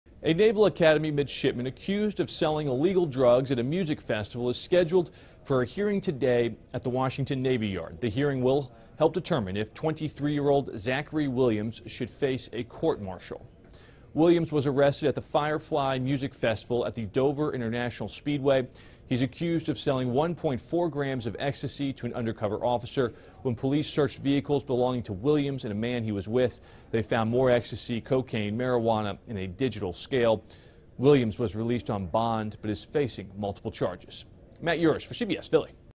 Naval Academy Midshipman Accused Of Selling Drugs At Firefly Music Festival
A hearing has been scheduled for a midshipman accused of distributing illegal drugs, including cocaine, at a music festival in Delaware. Matt Yurus reports.